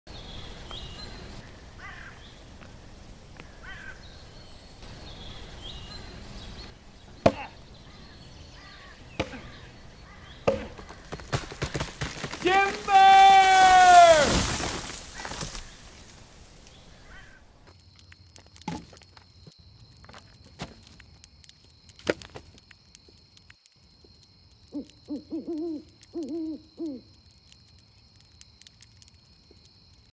Roger Levesque builds a bonfire ahead of Sounders-Timbers
Seattle Sounders FC legend Roger Levesque prepares for Sounders-Timbers by building a bonfire.